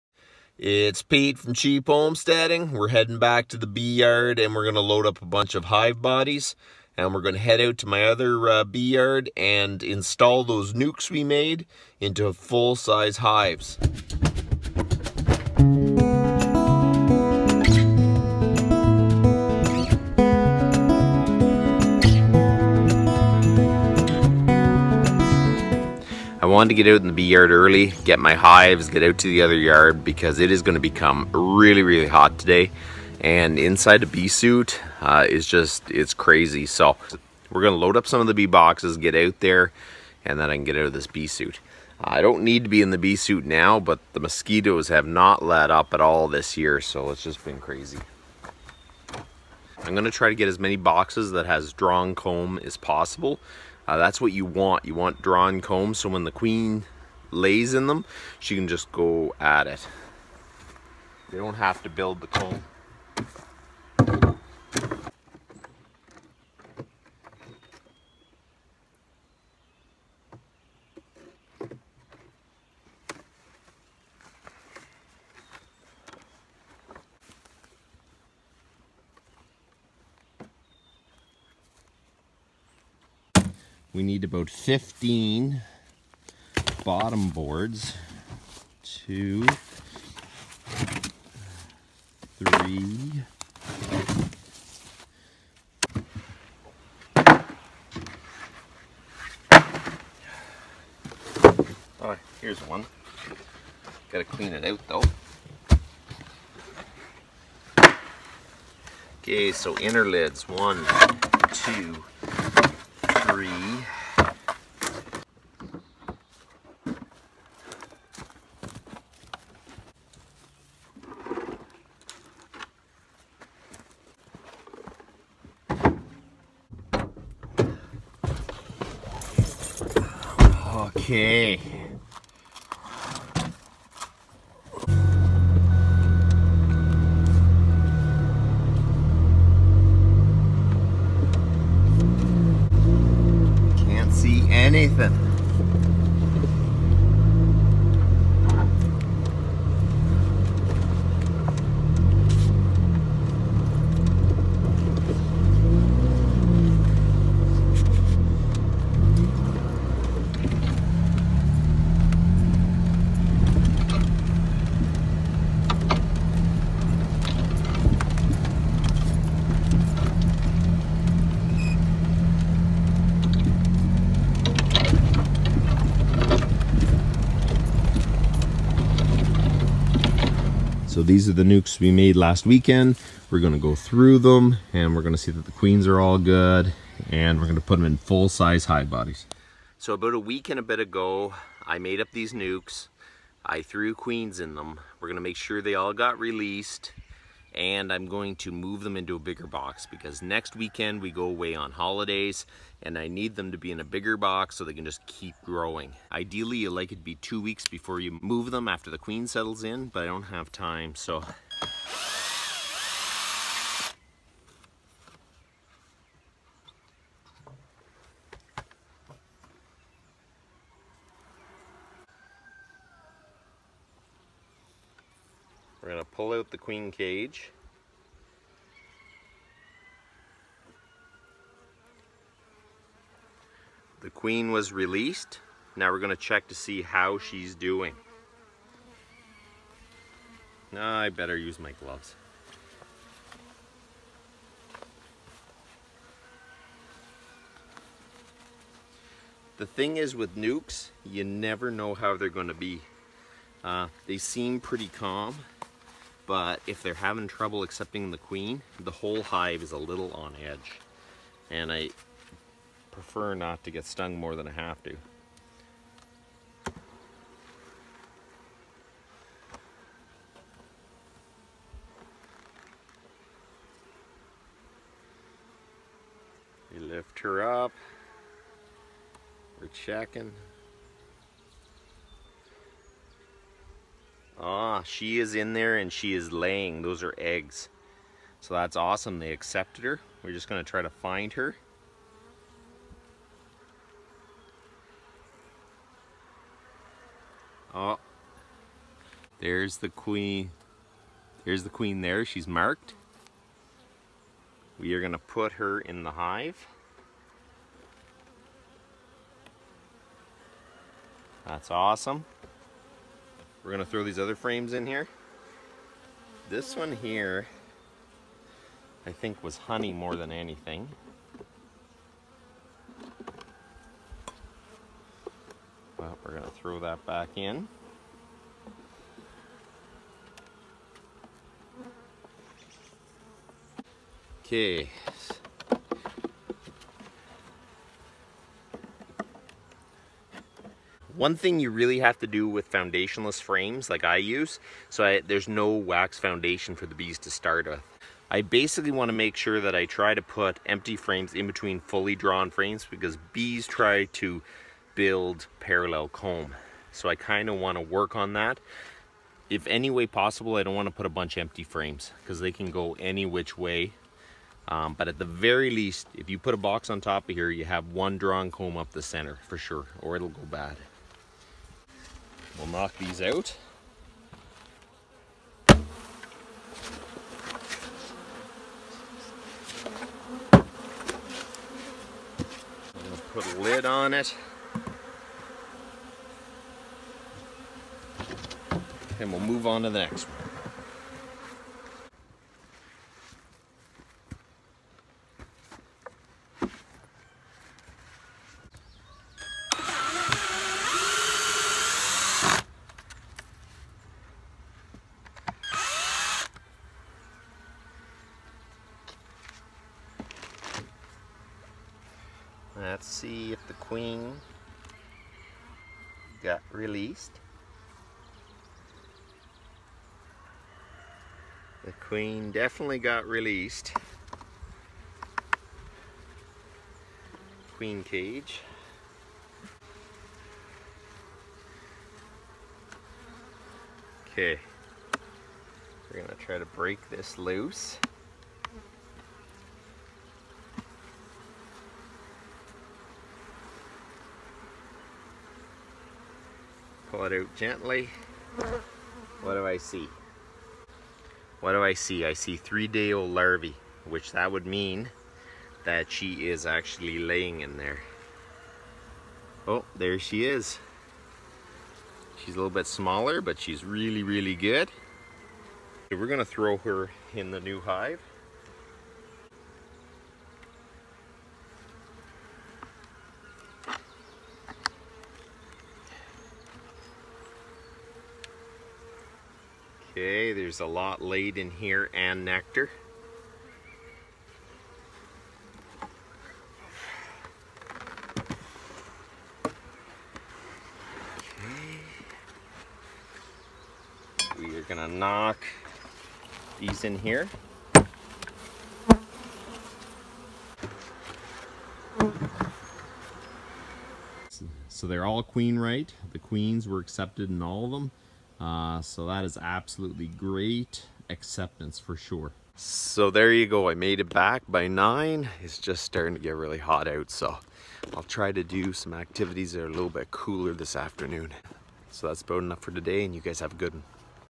Checking to see if the queens started to lay in the nucleus colonies
Cheap homesteading is installing his nucleus colony into full-size hives and checking if the queen has started to lay.